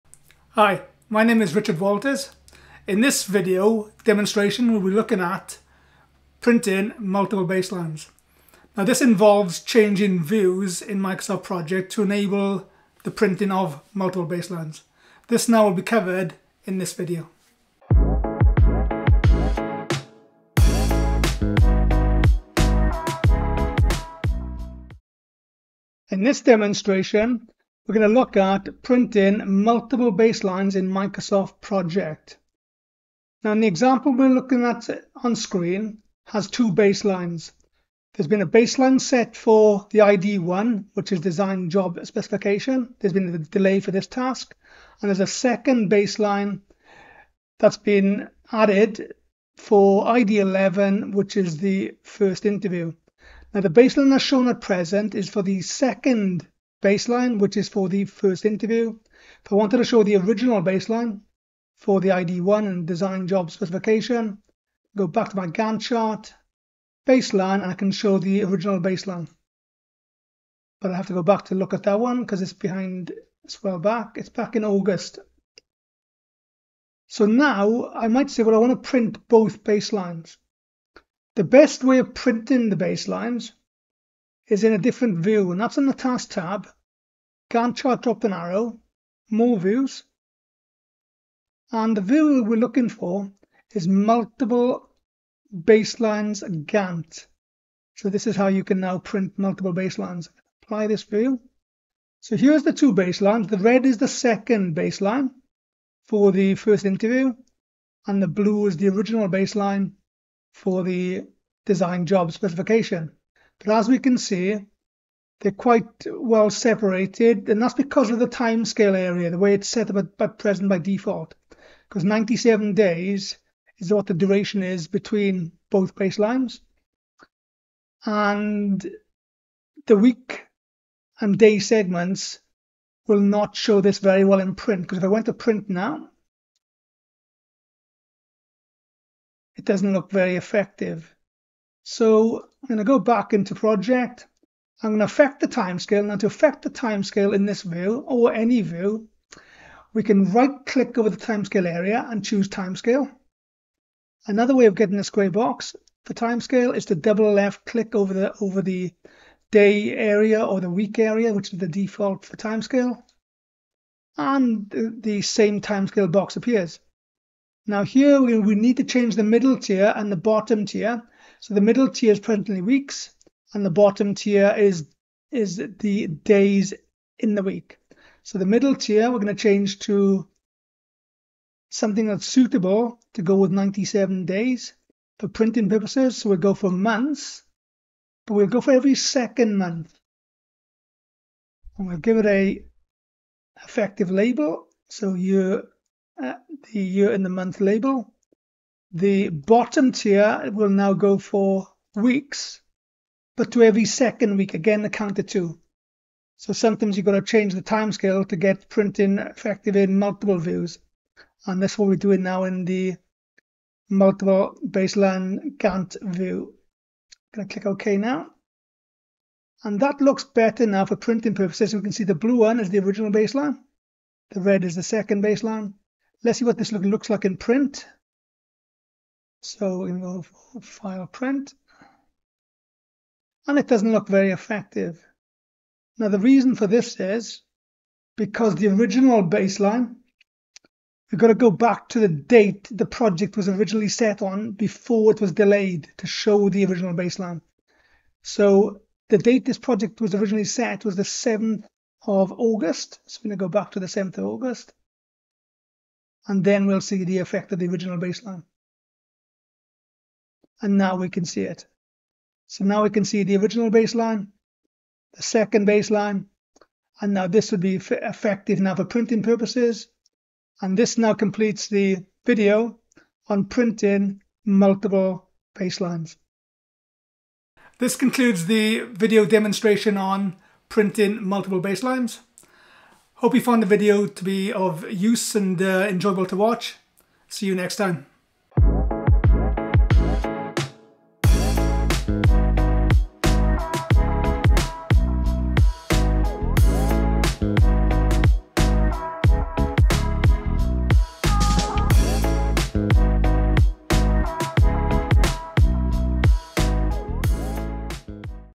HOW TO Setup and Print Multiple BASELINES in Microsoft Project
Today's video is all about printing projects which have multiple baselines. We will show you to how print a project in the format of a Multiple Baselines Gantt. We will also show you the optimal print settings to format your printing correctly.

⌚TIMESTAMPS:
00:00 - Intro
00:25  - Multiple Baselines Intro
01:20 - Printing Multiple Baselines
02:25 - Adjusting Project Timescales
03:50 - Printing Setup for Multiple Baselines